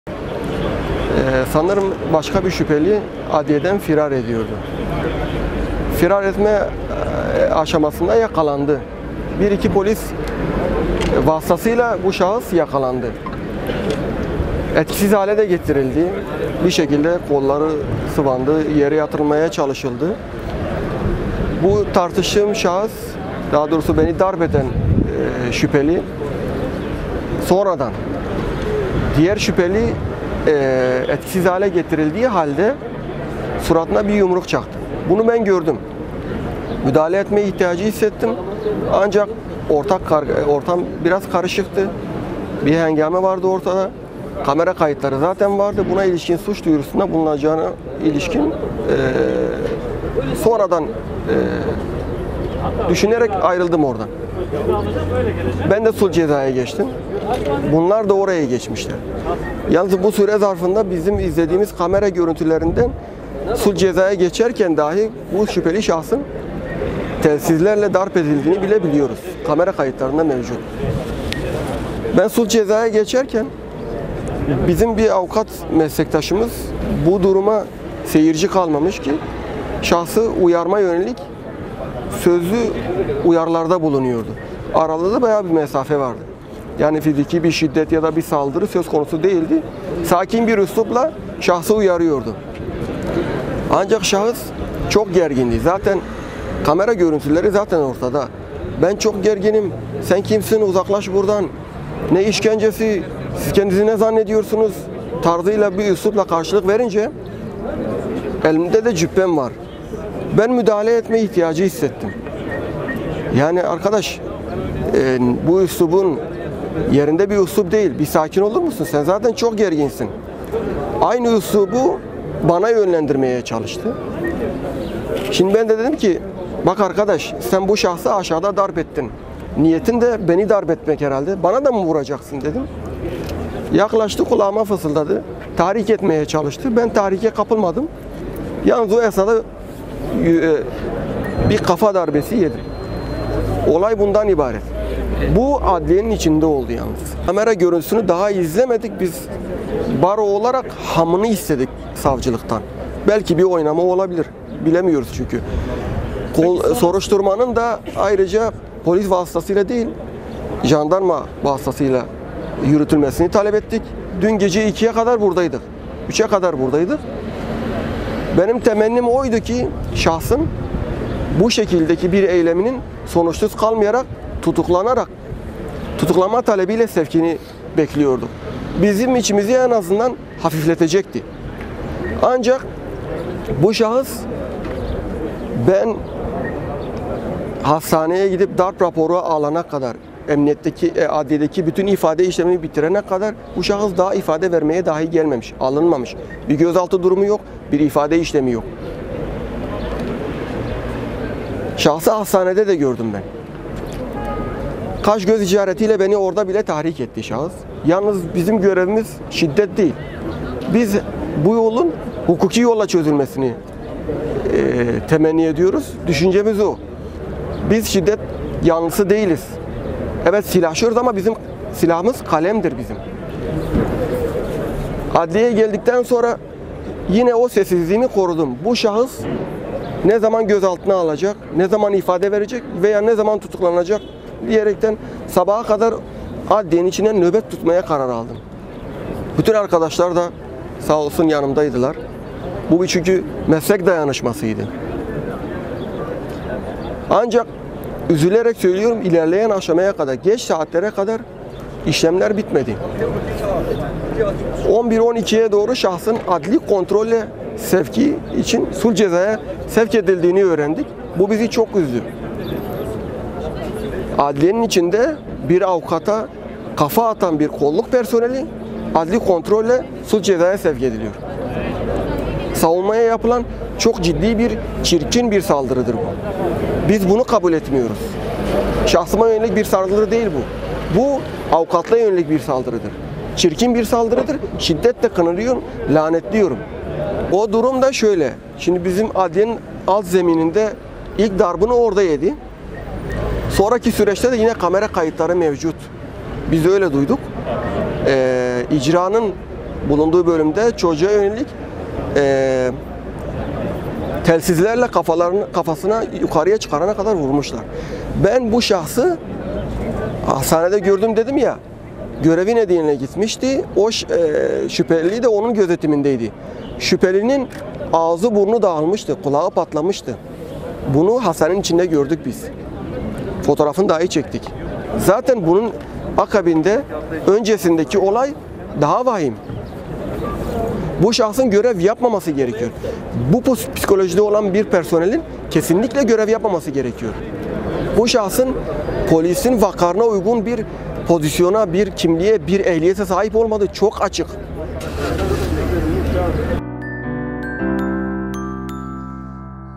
ADLİYEDE POLİSİN ŞİDDET UYGULADIĞI AVUKAT ORHAN ALPHAN KONUŞTU
Hem kendisine hem de tutukluya şiddet uygulayan polisin delilere ve tanık ifadelerine rağmen serbest bırakılmasına tepki gösteren avukat Orhan Alphan, olayın peşini bırakmayacağını söyledi.